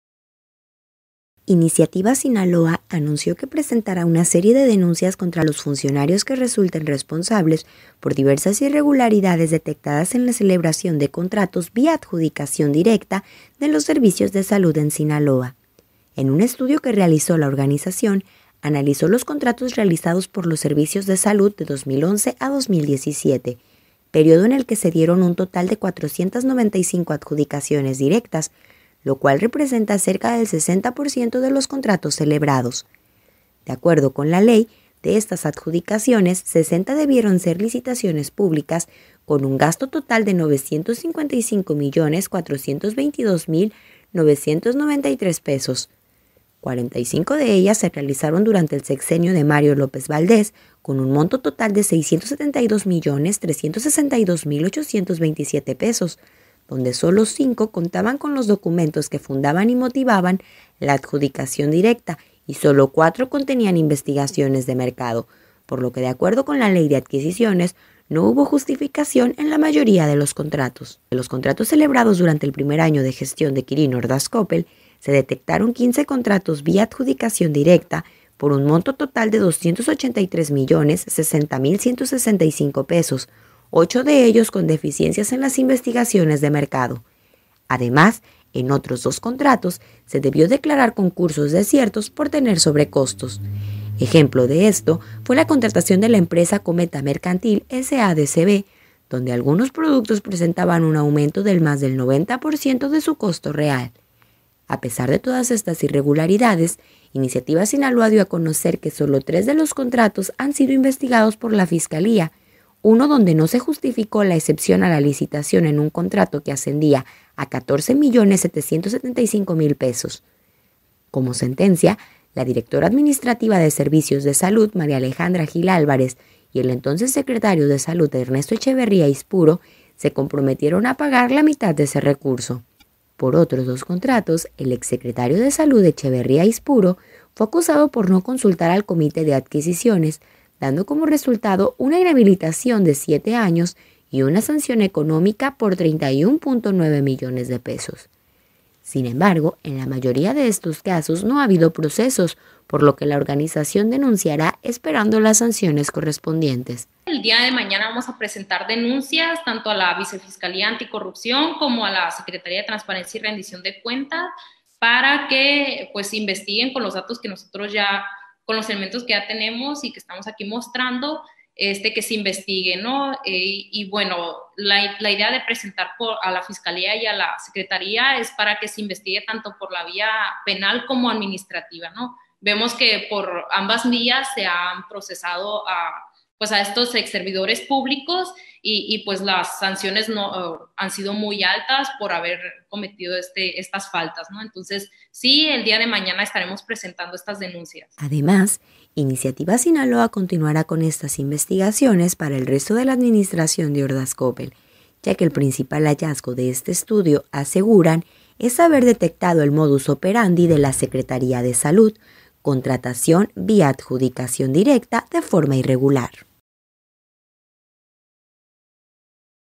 Detectan irregularidades en adjudicaciones directas de los servicios de salud
MMCuliacán Iniciativa Sinaloa anunció que presentará una serie de denuncias contra los funcionarios que resulten responsables por diversas irregularidades detectadas en la celebración de contratos vía adjudicación directa de los servicios de salud en Sinaloa.